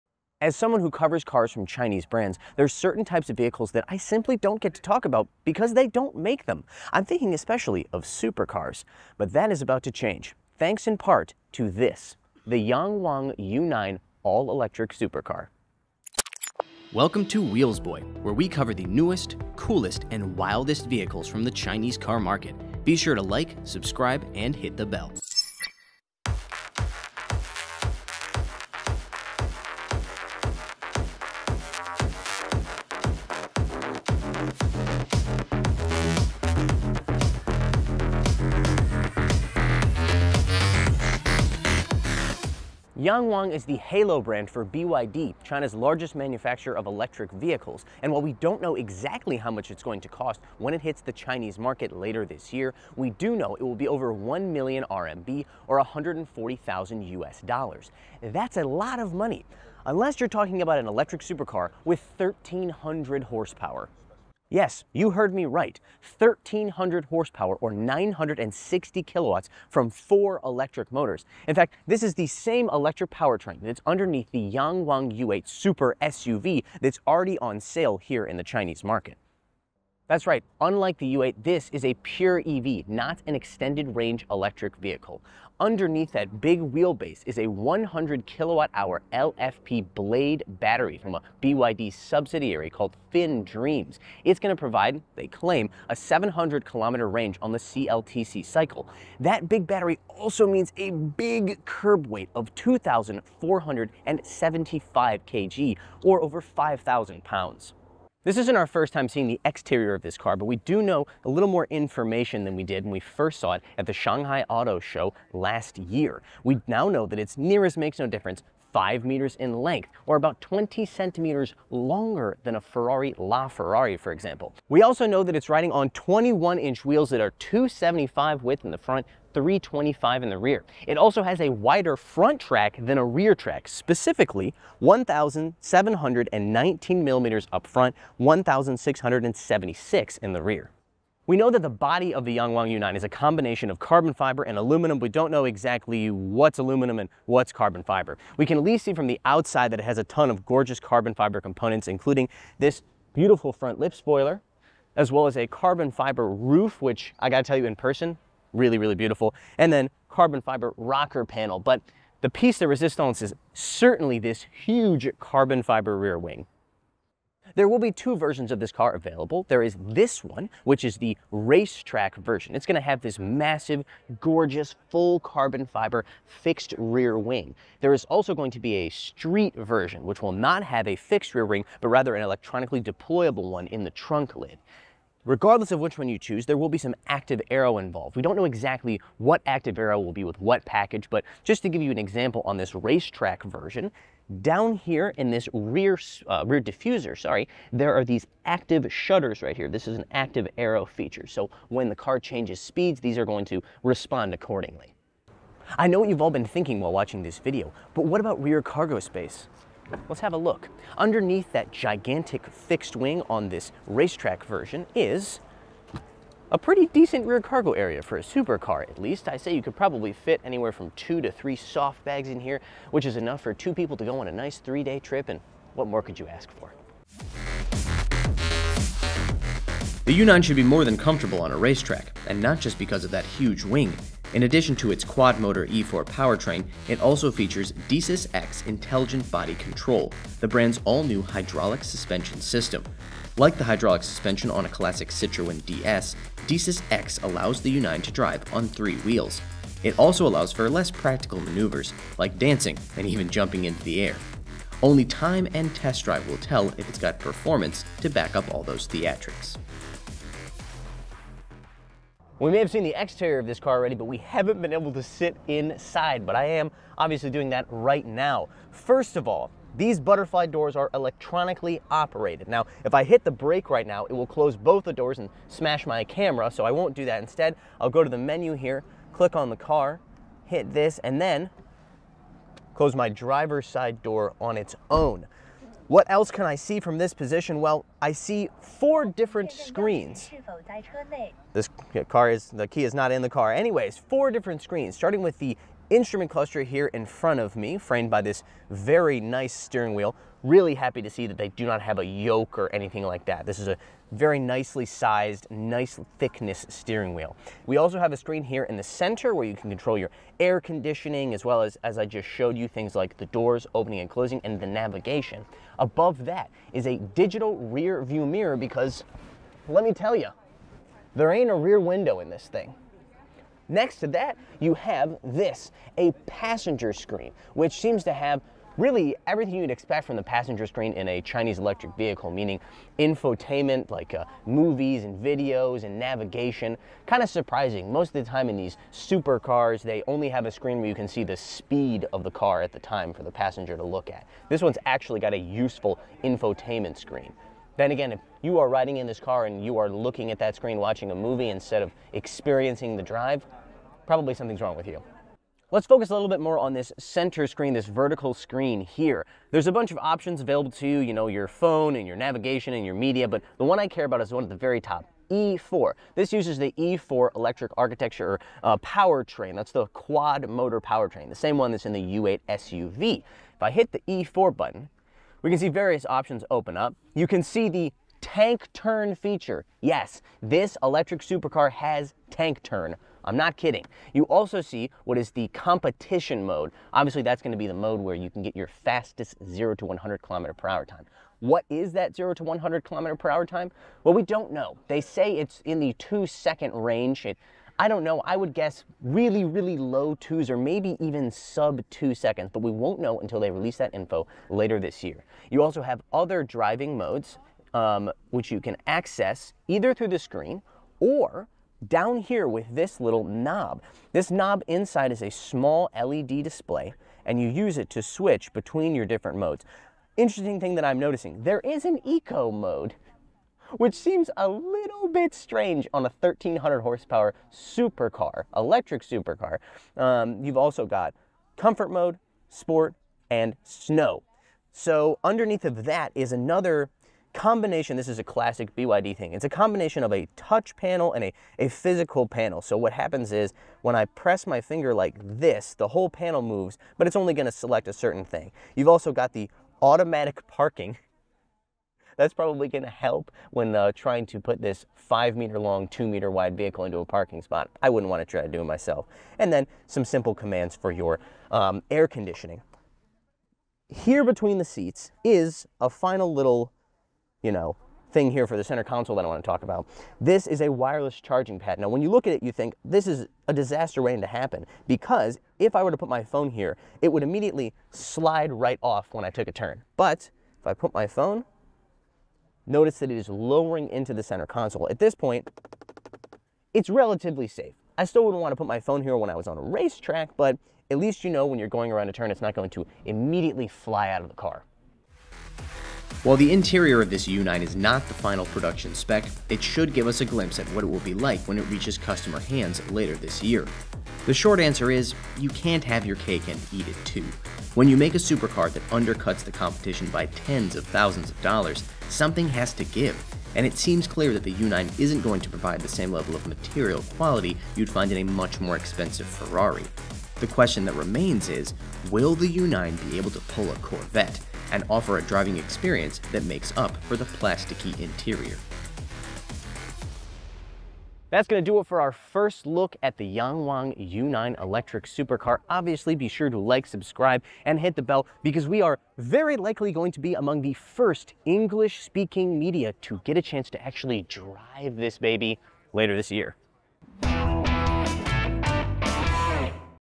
This 1300 HP Chinese Electric Super Car Is Coming For Ferrari
The Yangwang U9 is a quad motor electric monster with hydraulic suspension and something to prove.